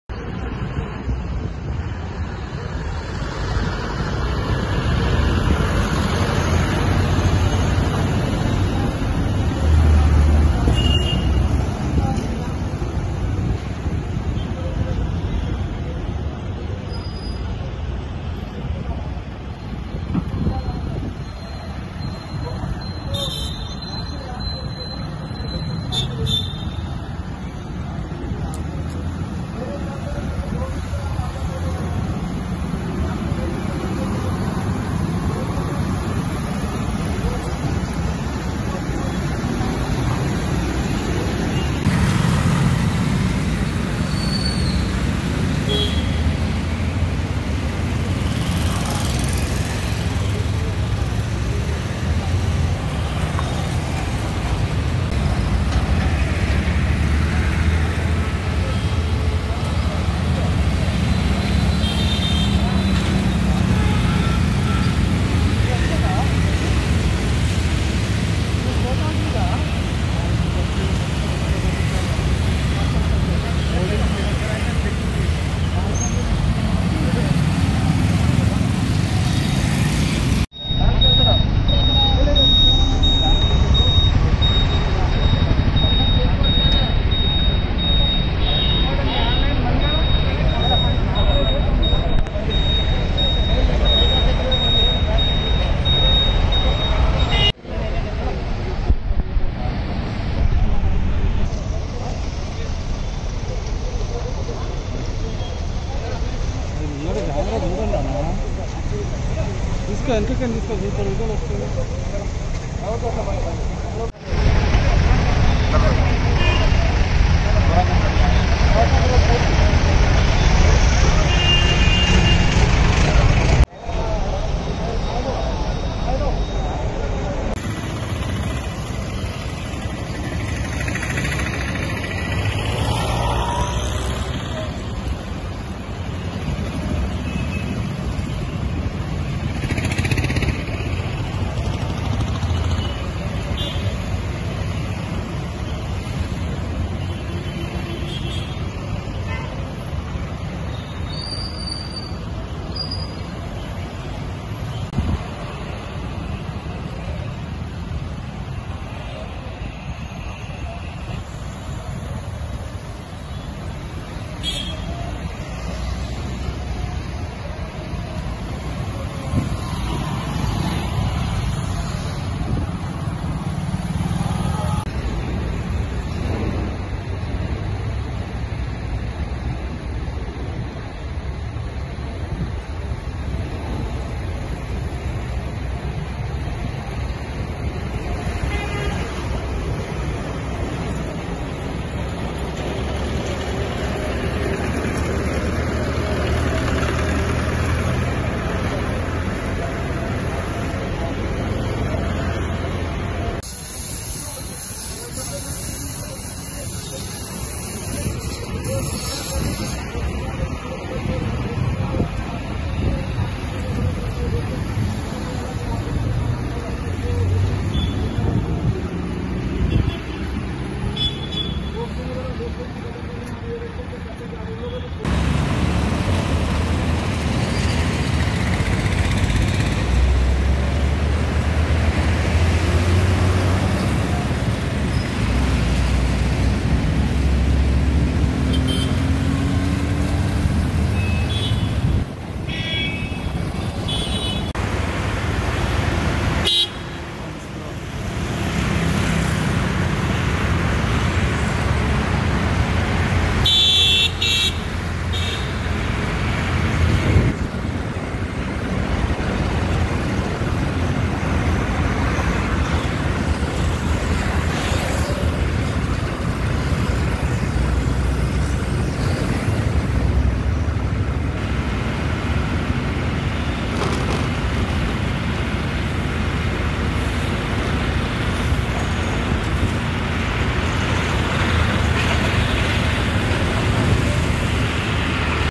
సైబరాబాద్ రోడ్లపై గర్జించిన సింగం ( సీపీ సజ్జనార్ ) | Sajjanar Strong Warning To Lockdown Violaters
సైబరాబాద్ రోడ్లపై గర్జించిన సింగం ( సీపీ సజ్జనార్ ) | Sajjanar Strong Warning To Lockdown Violaters | Cyberabad Police | NEWS16 Telugu